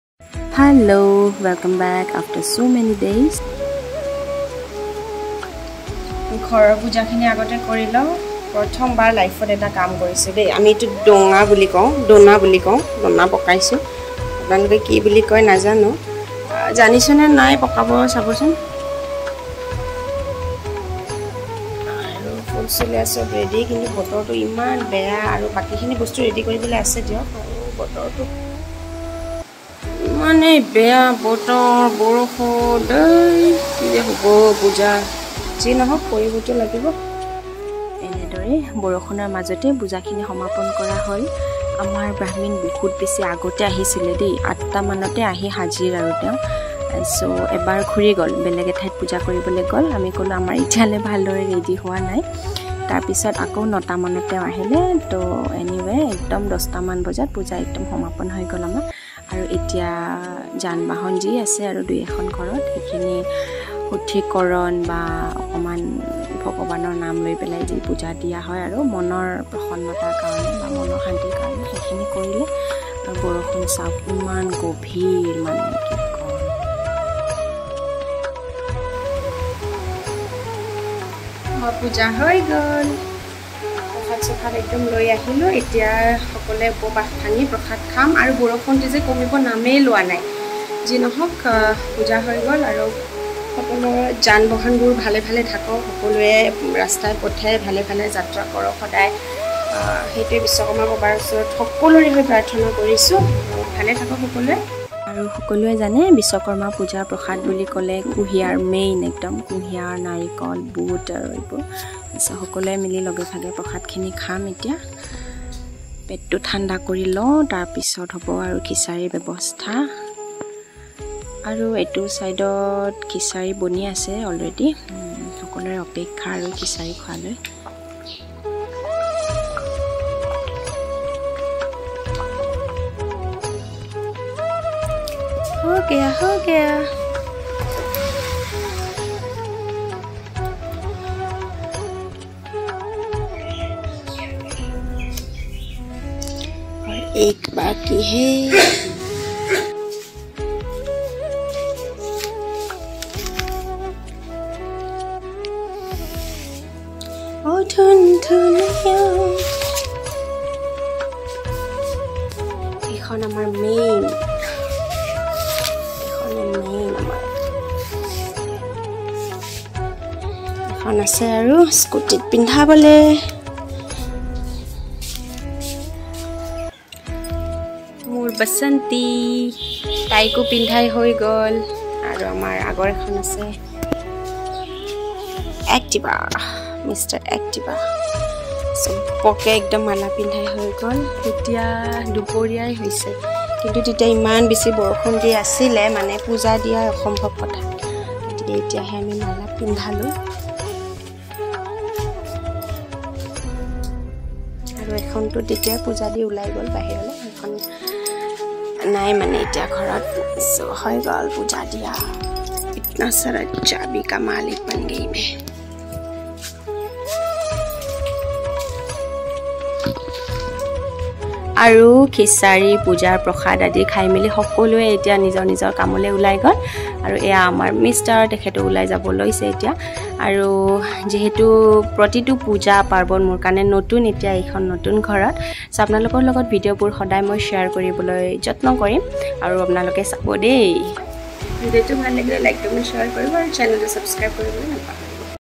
Viswakarma Puja at Home | Kokrajhar
Here's my short Vlog on Viswakarma Puja at Home !

Please Do Watch, You'll Enjoy this 😍🌟❤️

Drive And Ride Safe Everyone! 🥰🌼